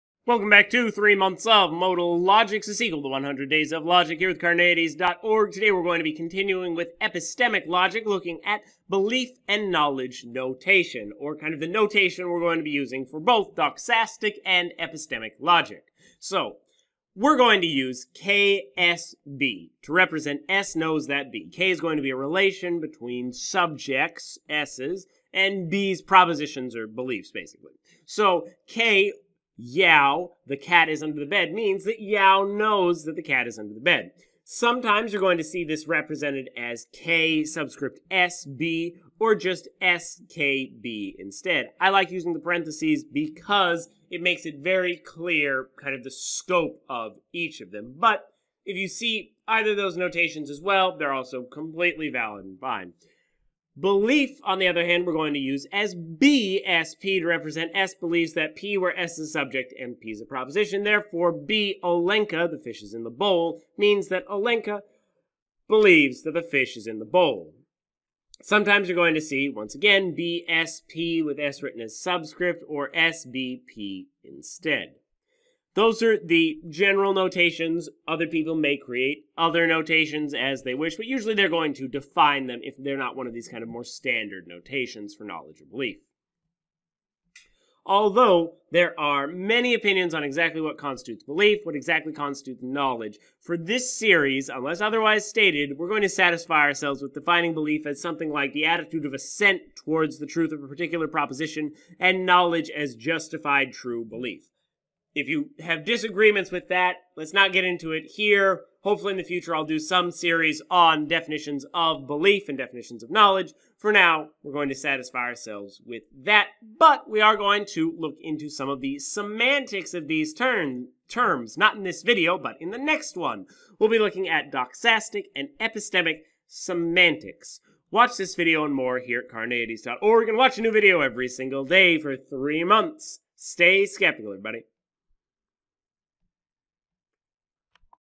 Belief and Knowledge Notation (Epistemic Logic)
An explication of the notation for belief and knowledge
B(s)(p) : A subject s believes that p
K(s)(p) : A subject s believes that p